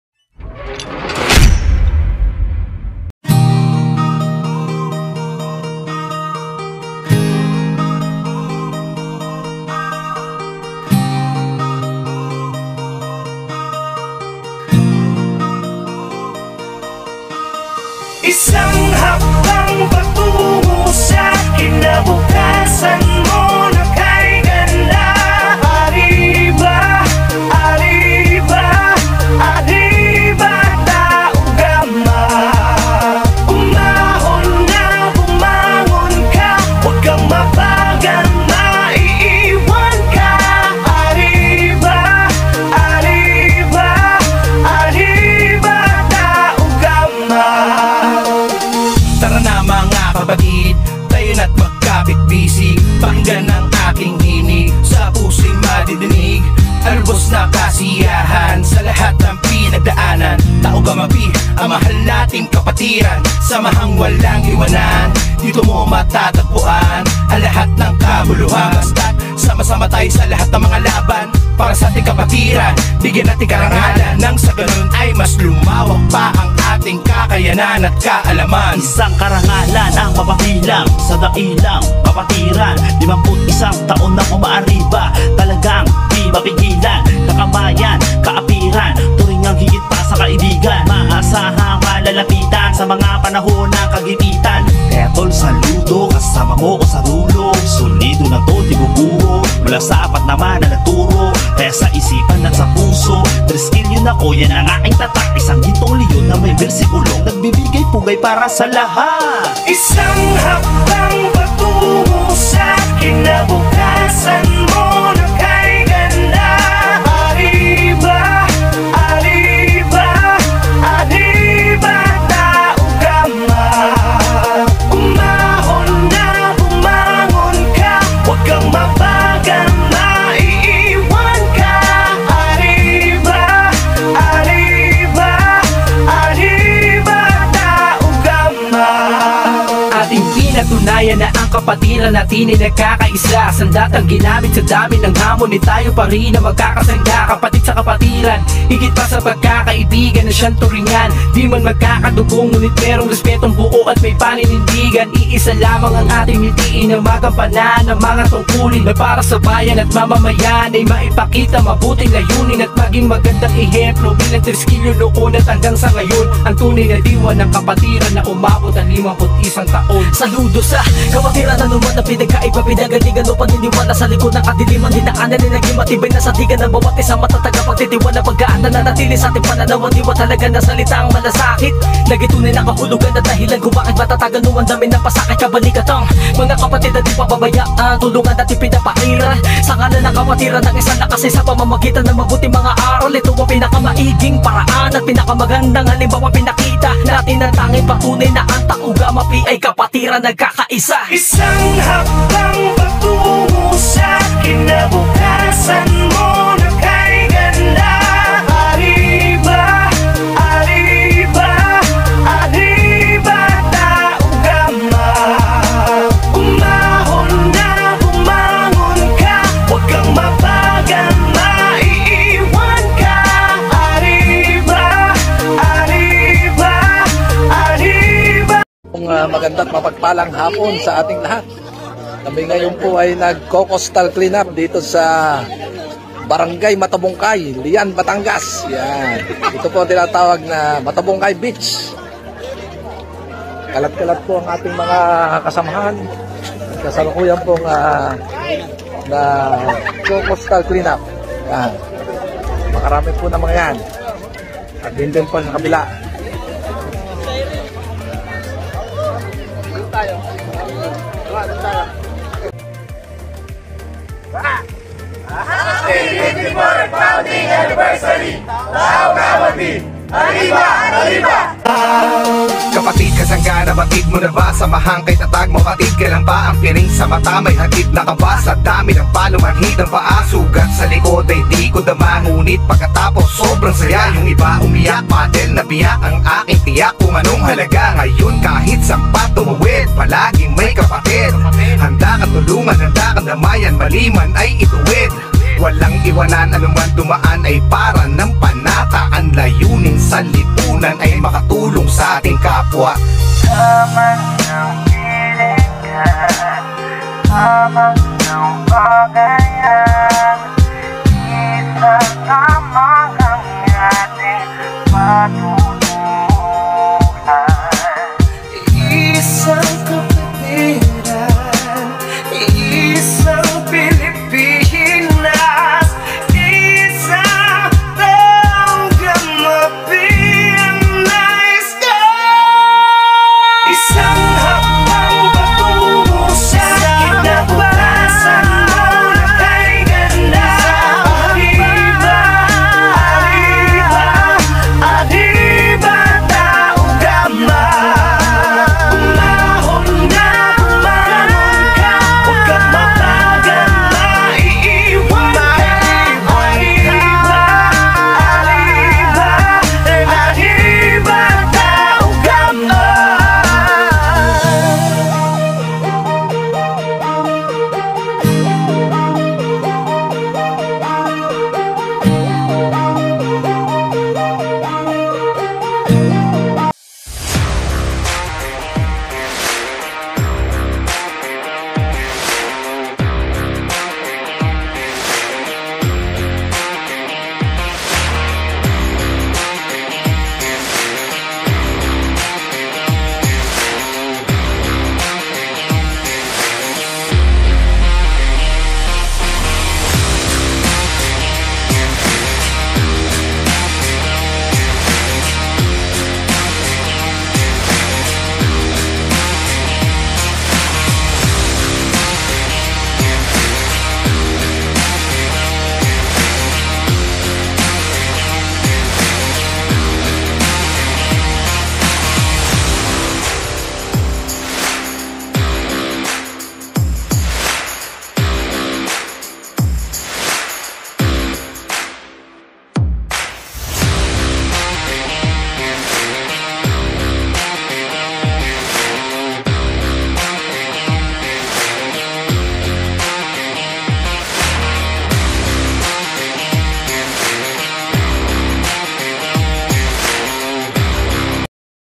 Triskelions' Grand Fraternity Coastal Clean up @Matabungkay, Lian,Batangas 54th Founding Anniversary
"TΓISKΣLIΦN"